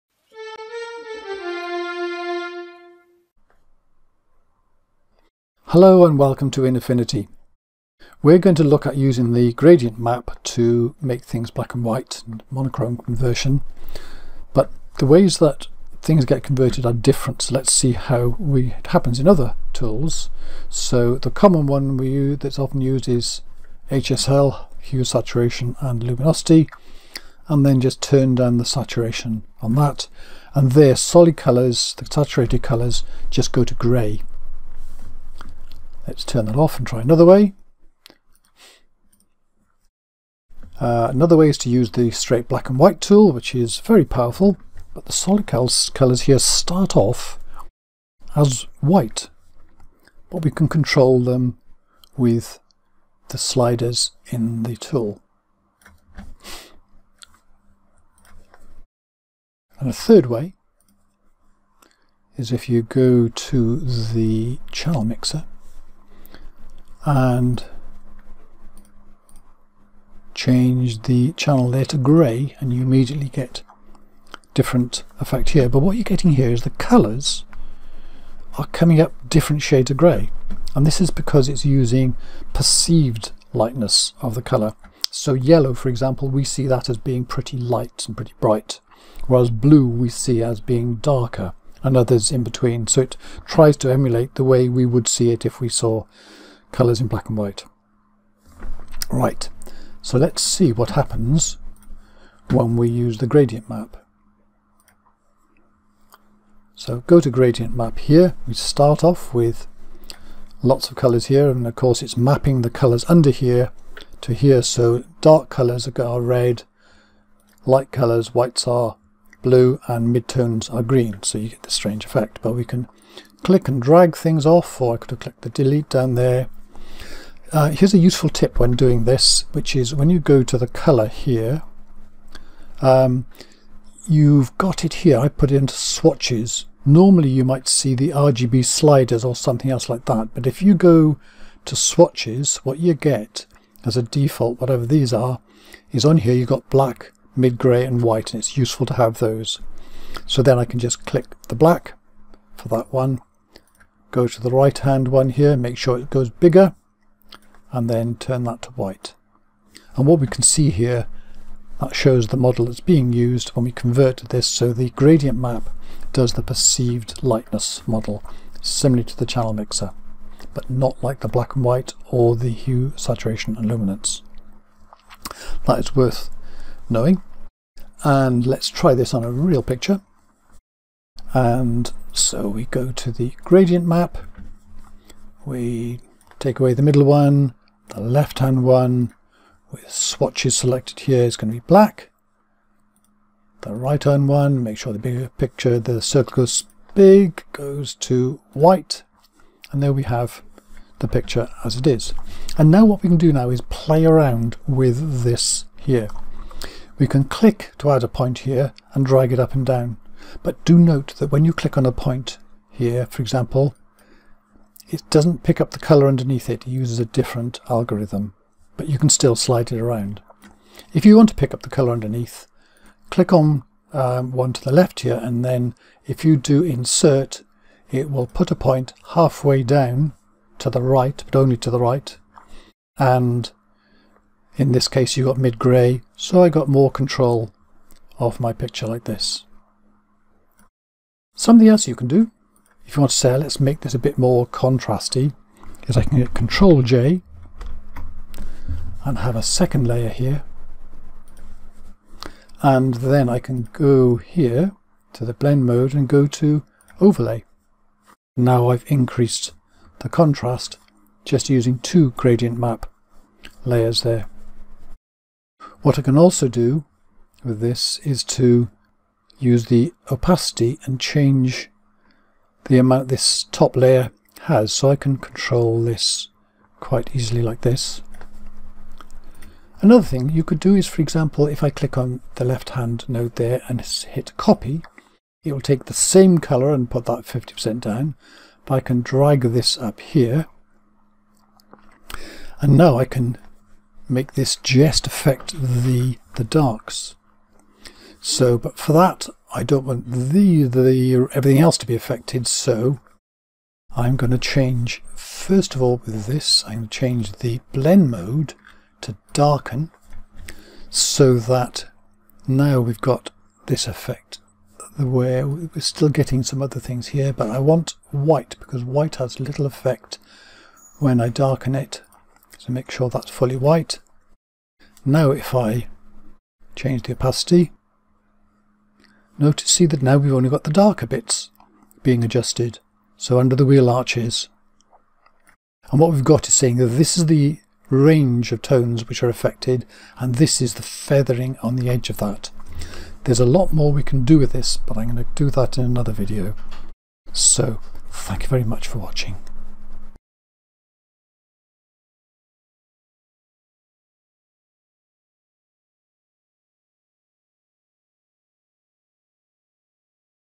Monochrome Conversion using the Gradient Map (using Affinity Photo)
The Gradient Map tool offers a unique way to convert a colour photo to black and white and then to edit it in various ways. Here's the basics of how to do this using the excellent Affinity Photo.

The web page for this video is here (with links to related videos)